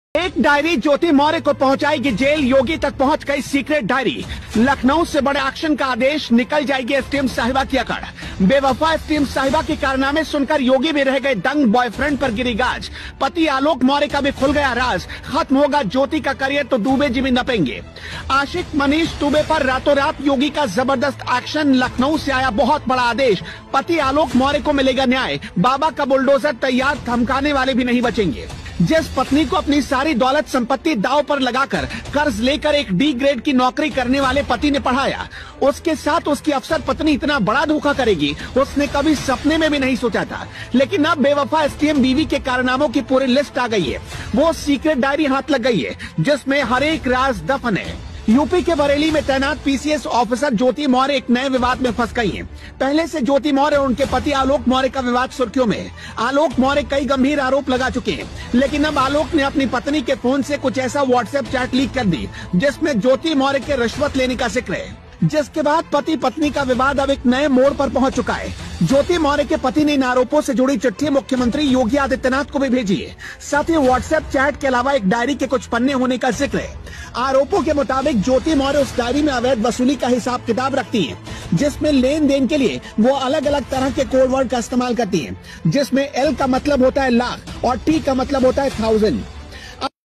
ज्योति मौर्य को हटा दिया एसडीएम SDM पदसेjyoti morya ko hata dia SDM pad se#shorts#yogiadityanath
ज्योति मौर्य को हटा दिया एसडीएम SDM पद से || jyoti morya ko hata dia SDM pad se
Jyoti morya ki kia SDM pad se nirast